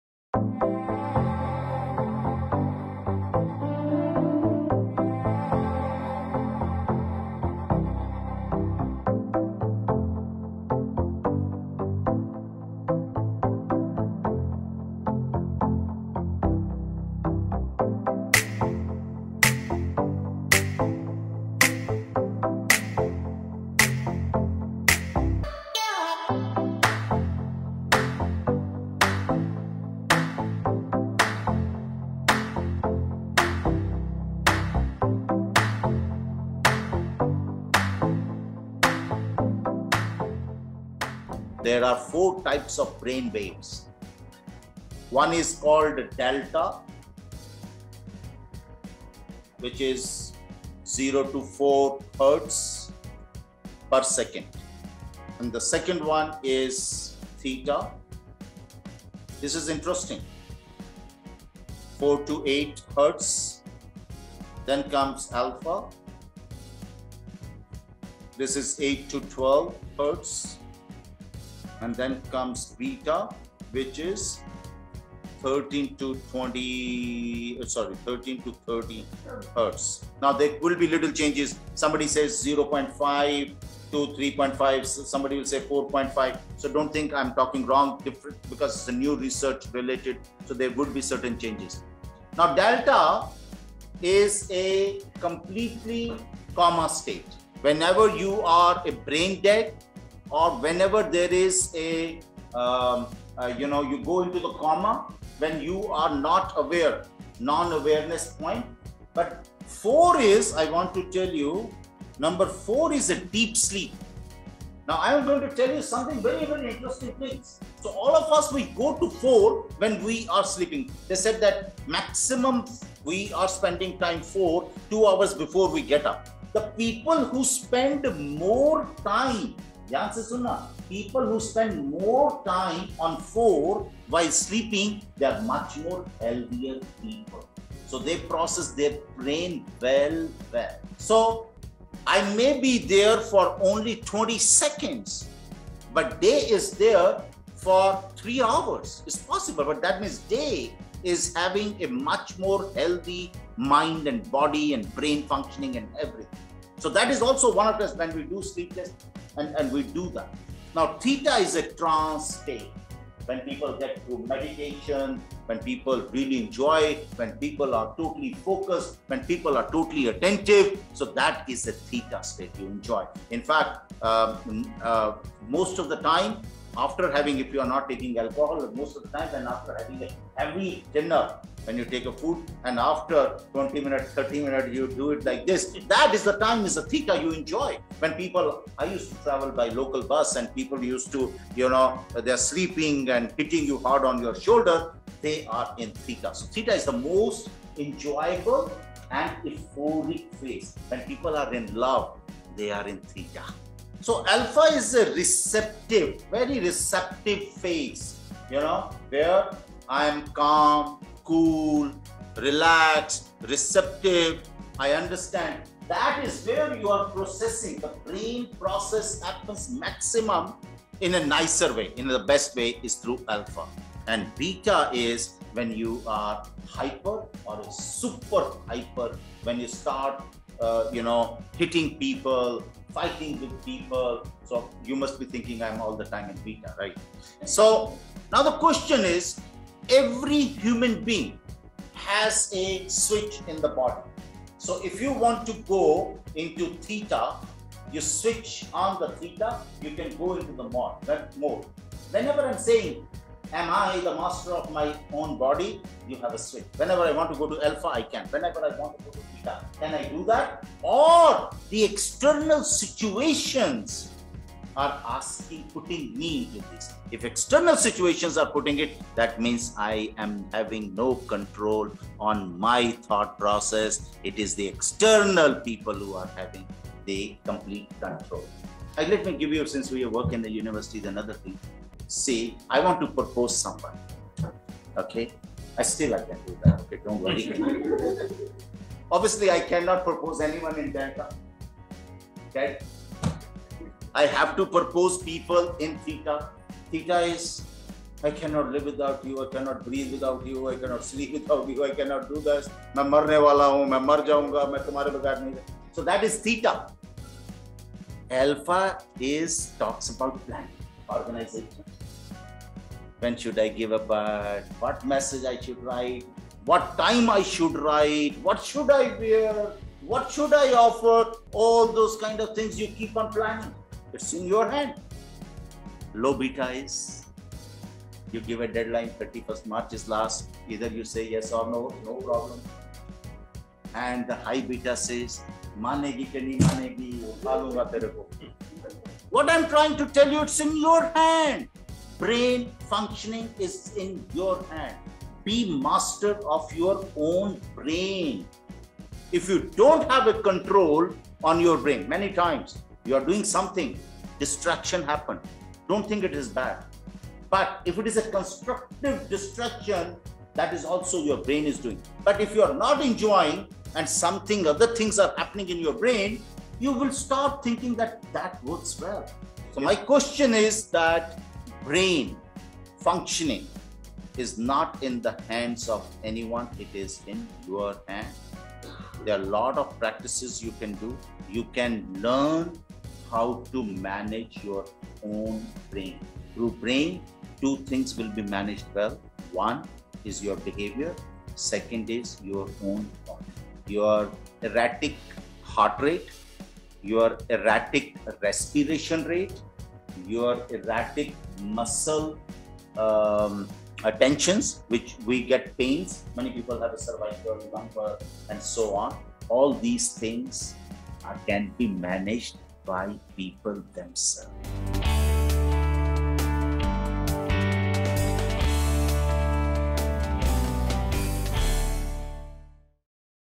Brain Waves and Our Behaviour
Do you know that the most complex organ of the human body i.e. brain emits different types of brain waves that impact our behaviour? These waves are of different frequencies and they determine our behaviour. For instance, the frequency increases while we get angry and subsides while we sleep. Learn more about these interesting cognitive functions in this short presentation by Dr. Sahni...